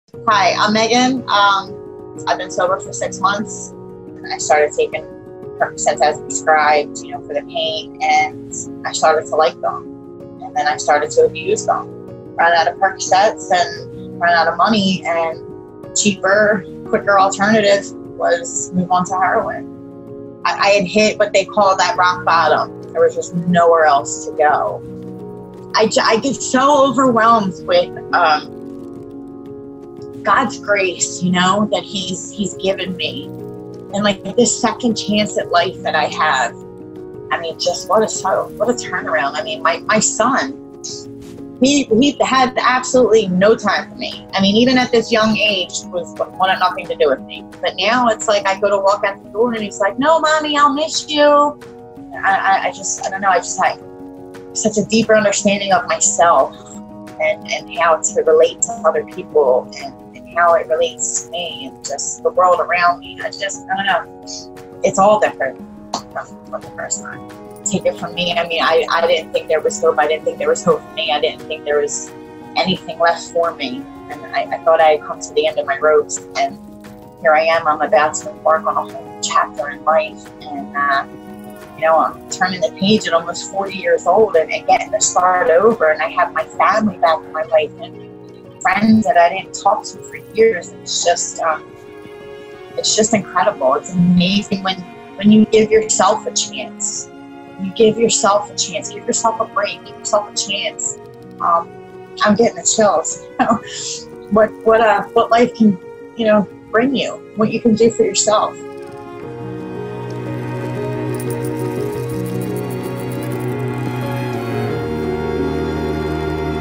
Megan
Megan has been in active rebirth for 6 months now and says she feels overwhelmed by God’s grace and the second chance that she now has in recovery. Though addiction once held her back, she gets to start over and turn the page to a whole new chapter of her life. Her friends and family are back in her life, and she’s grateful every day that she took those first steps towards recovery.